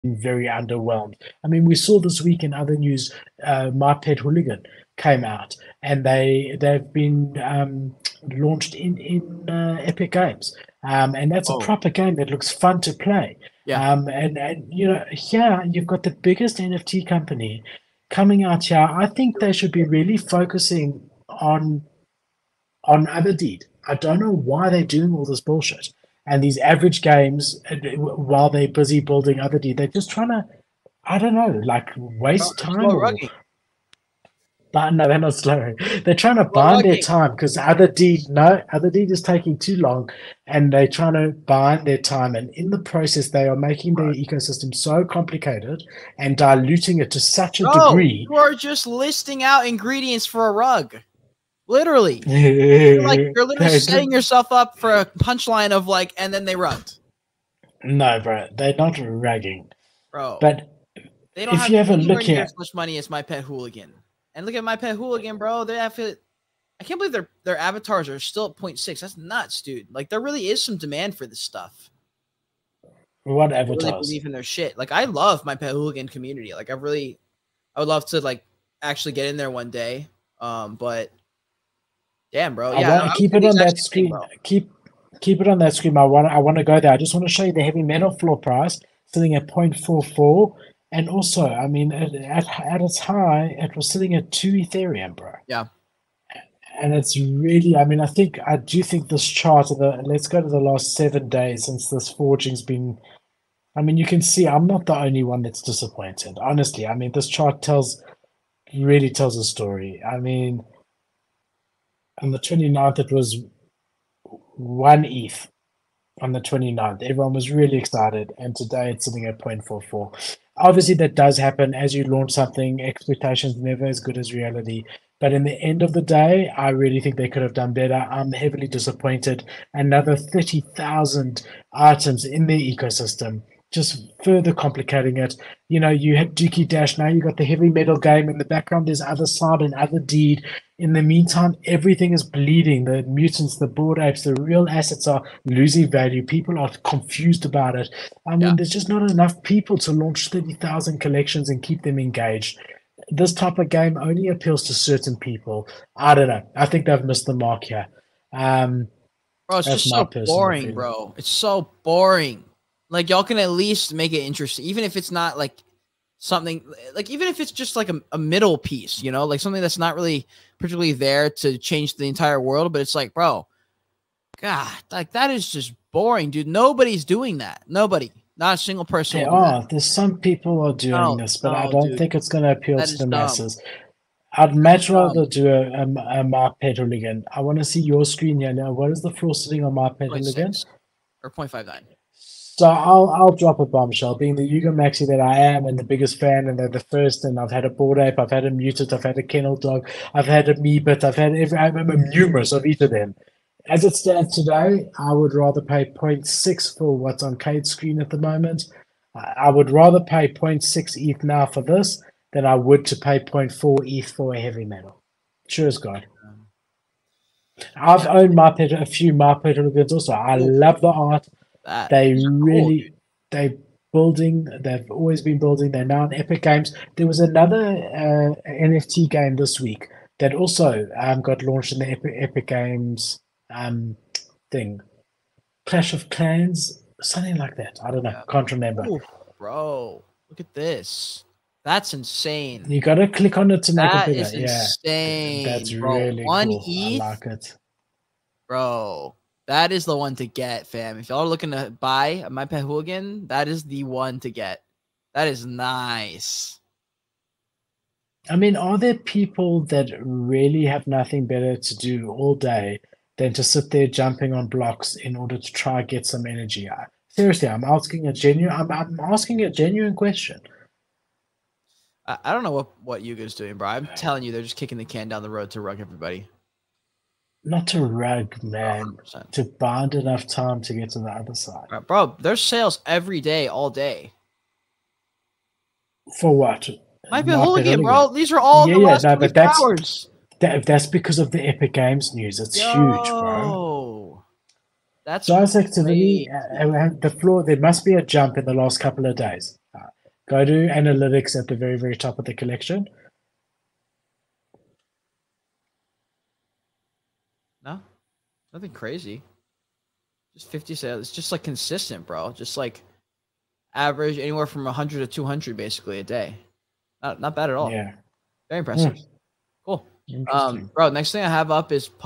My Pet Hooligan NFT GAME Launches On Epic Games Store
. HIGHLIGHTS:
🚨HV-MTL Forge
🚨Azuki Elementals
🚨FCBarcelona X WOW
🚨Pudgy Penguins
🚨Bodoggos
🚨Crypto Charts

⭐️THE WEB3 SHOW LINKS⭐️